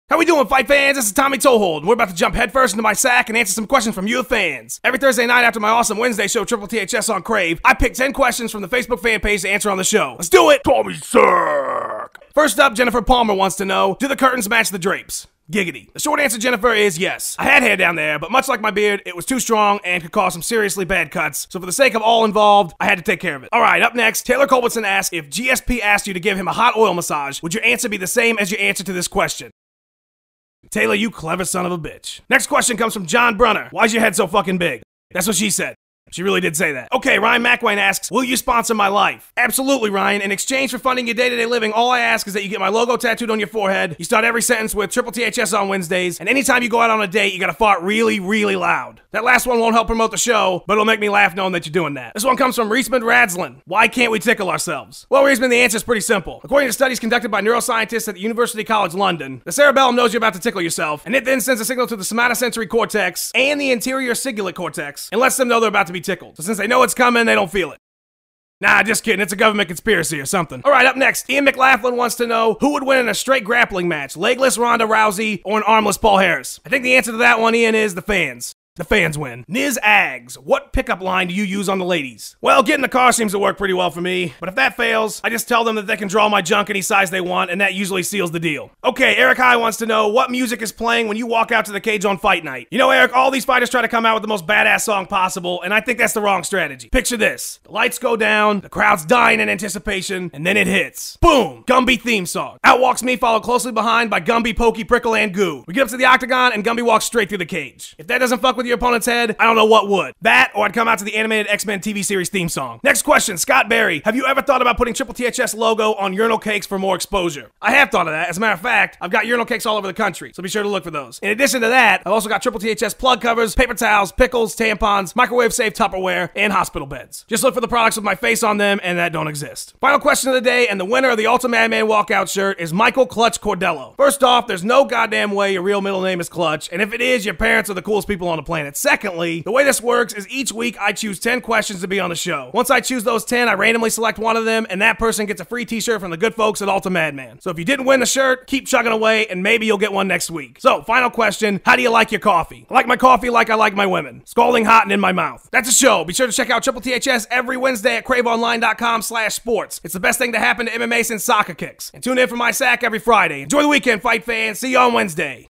TOMMY'S SACK 3!!! FAN Q&A!!!
What's up, fight fans!? This is my new Friday post-TTTHS Q&A session. Every Thursday night, I'll ask for questions on my Facebook Fan Page and answer 10 of them on 'Tommy's Mail Sack!'

One luck fan gets a free t-shirt from Ultimmadman!!!

NEWEST TTTHS!!! SHOGUN GASSED OUT!?!?!?